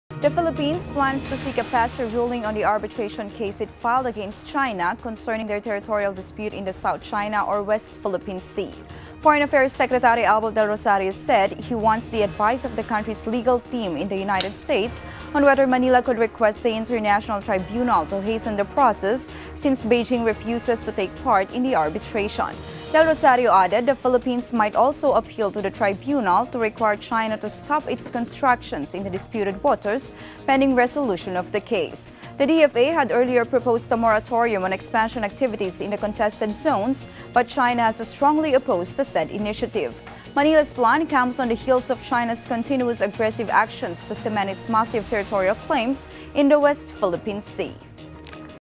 Manila seeks faster ruling on Maritime dispute with China
The Philippines aims to seek a faster ruling on the arbitration case it filed against China concerning the territorial dispute in the West Philippine Sea.